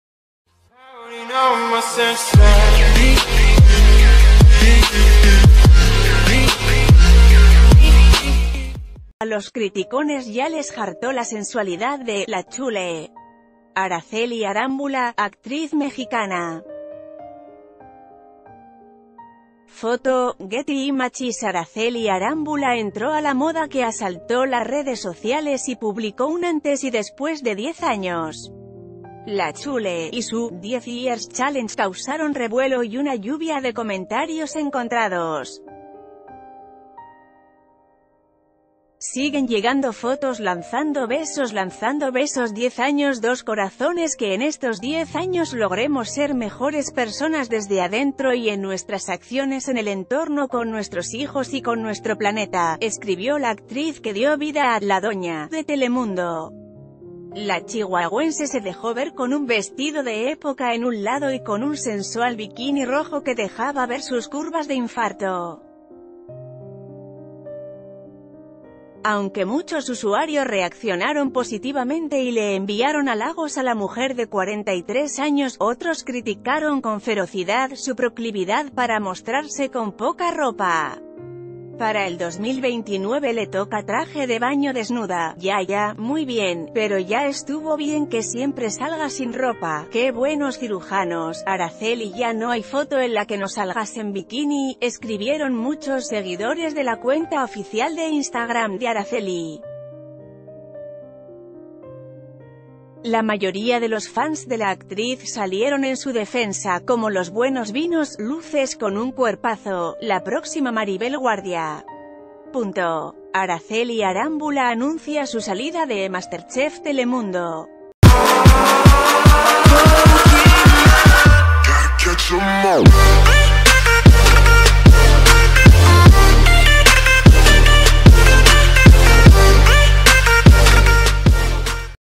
Aracely Arámbula hizo su “10 Years Challenge” en bikini y las críticas se la acabaron
A los criticones ya les hartó la sensualidad de "La Chule" Aracely Arámbula, actriz mexicana. La “Chule” y su “10 years challenge” causaron revuelo y una lluvia de comentarios e...